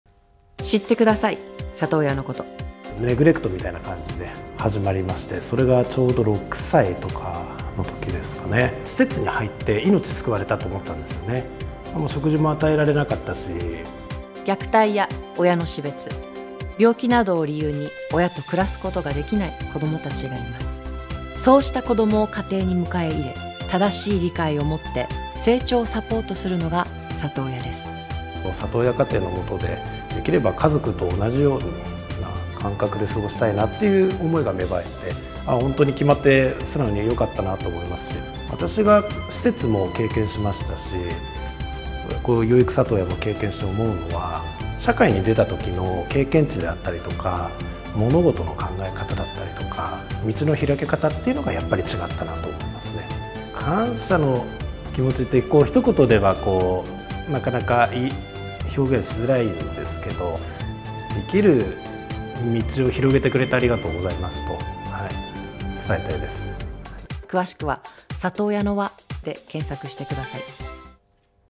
【PR】厚生労働省 令和4年度里親制度広報啓発動画
『普通の人が普通に里親になる』社会を目指して。それぞれの事情で親と離れて暮らす子どもたちが、日本には約4万2千人います。そうした子どもを自分の家庭に迎え入れ、さまざまなサポートを受けながら養育するのが「里親制度」です。
冨永愛さん、佐藤浩市さんのインタビューのほか、里親の方などのインタビュー記事をまとめた特設ページを朝日新聞GLOBE+で展開していますので、こちらもご覧ください。